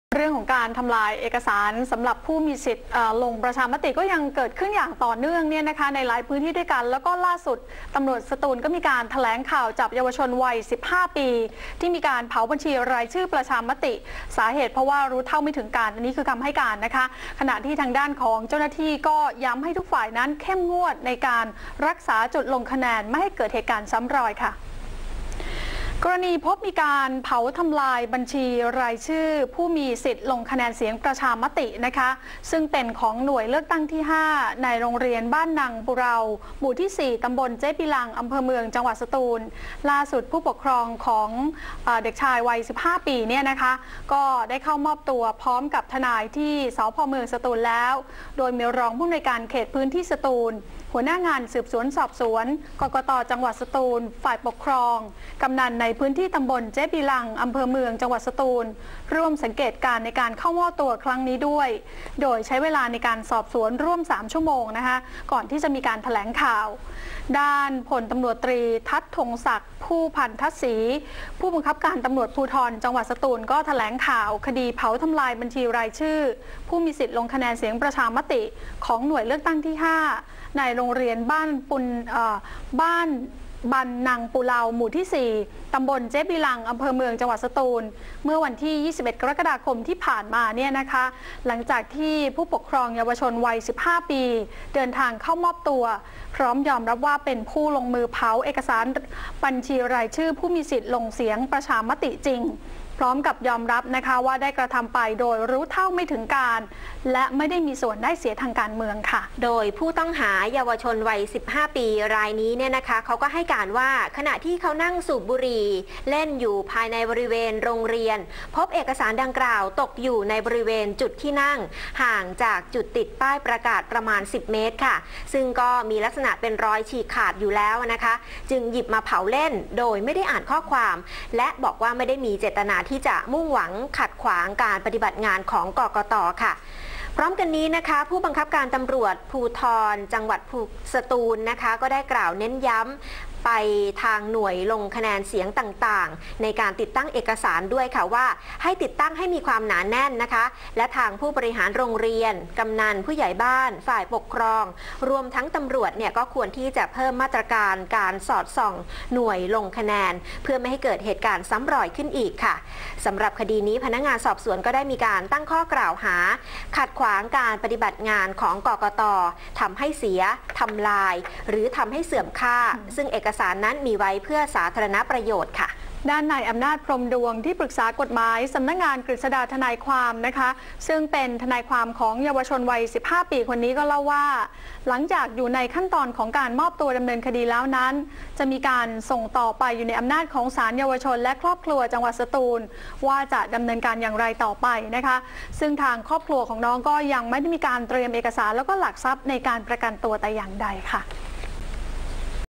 เยาวชนชายวัย 15 เผาบัญชีรายชื่อประชามติ เหตุรู้เท่าไม่ถึงการณ์
22/07/2016 เยาวชนชายวัย 15 เผาบัญชีรายชื่อประชามติ เหตุรู้เท่าไม่ถึงการณ์